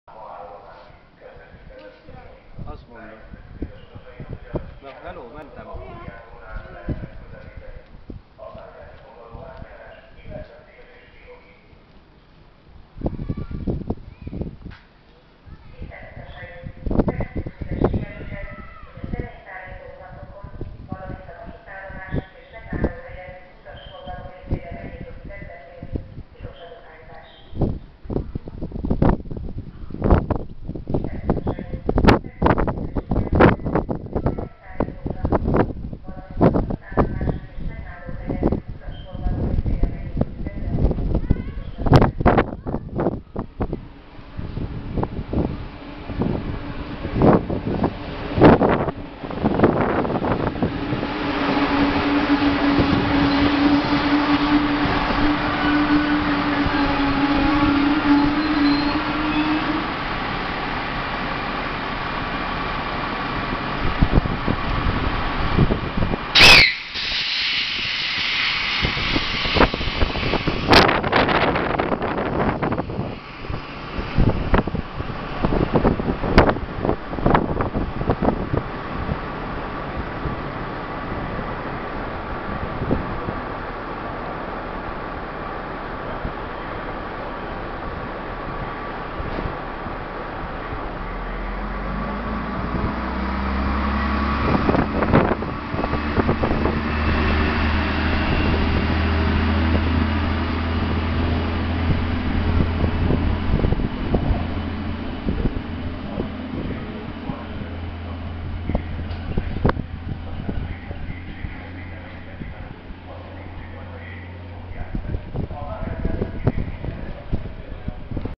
2017.03.04 Pápa személyvonat
Győr felől személy érkezik, Tatabánya felől ma 10 éve semmi.